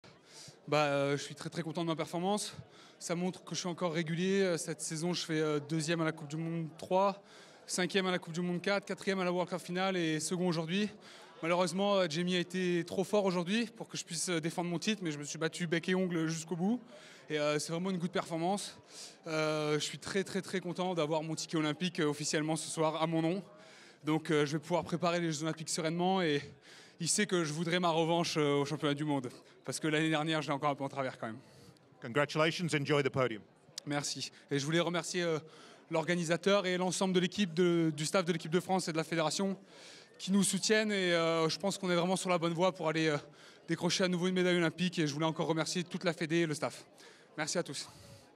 Prades «Je suis encore régulier» - Pentathlon moderne - Euro (H)
VIDEO PENTATHLON - Après le titre en 2018, l'argent. Valentin Prades a été devancé de 13 secondes par le Britannique James Cooke, champion du monde en titre, à l'issue des épreuves de natation, d'escrime, d'équitation et de laser-run (combiné course-tir), ce samedi à Bath, lors des Championnats d'Europe.